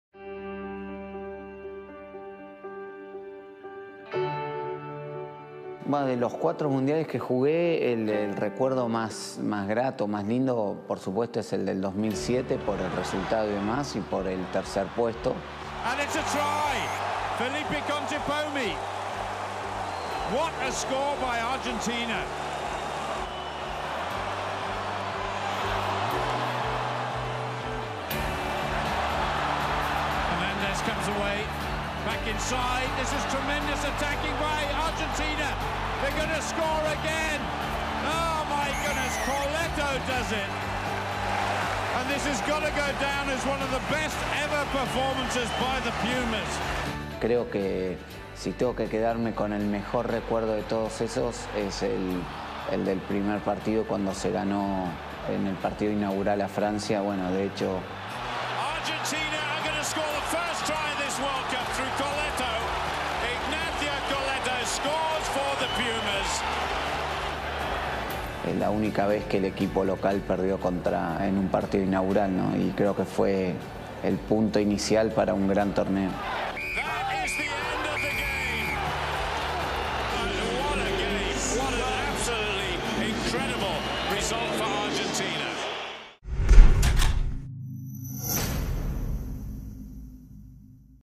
Felipe Contepomi | World Cup Memories
Argentinian legend Felipe Contepomi looks back on one of the greatest achievements of his career as The Pumas defied expectations to claim third place at the 2007 Rugby World Cup.